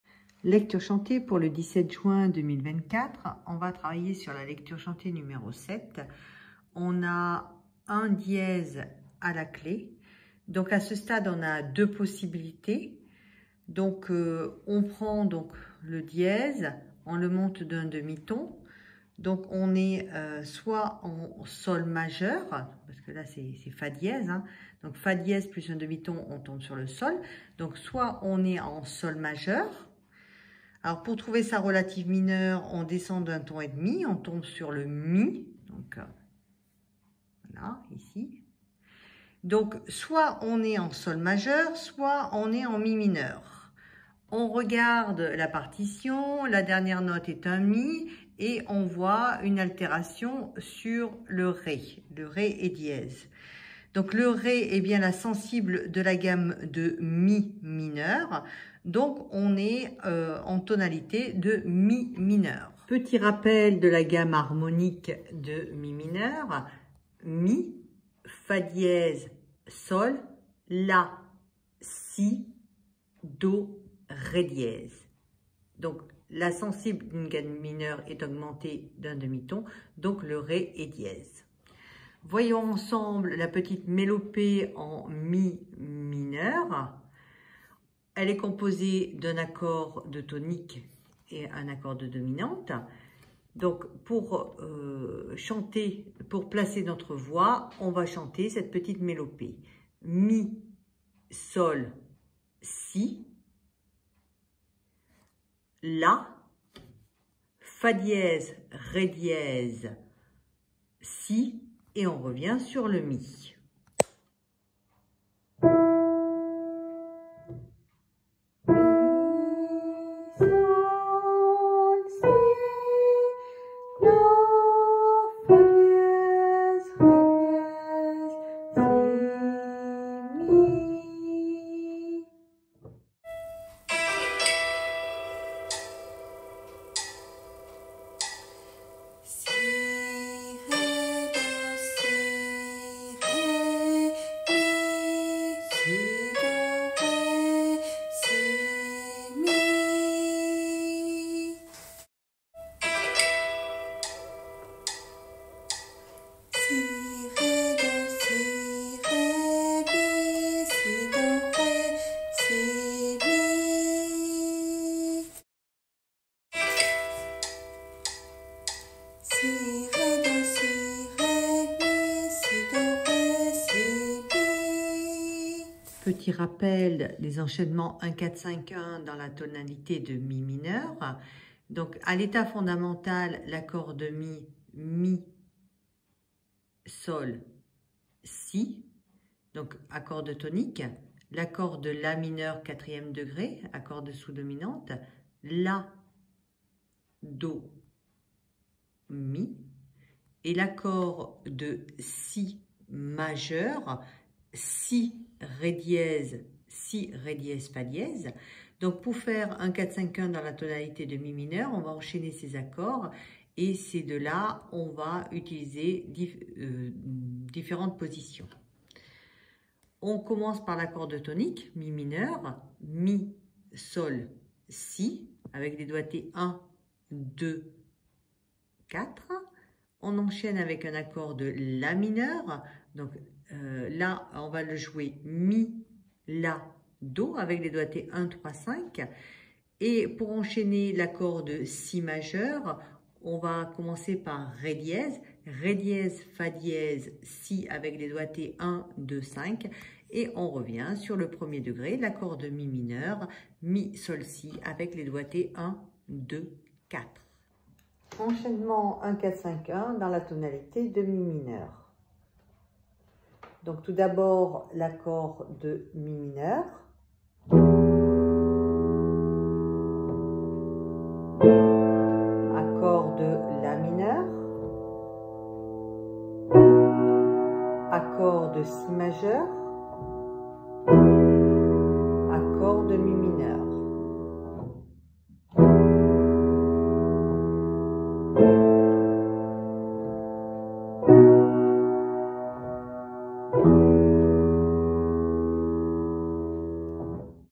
Voix chantée : LC7 du 17/06/24 en Mi mineur 3 tempos et enchaînements I IV V I
Préparation cours de FM du 17 juin 2024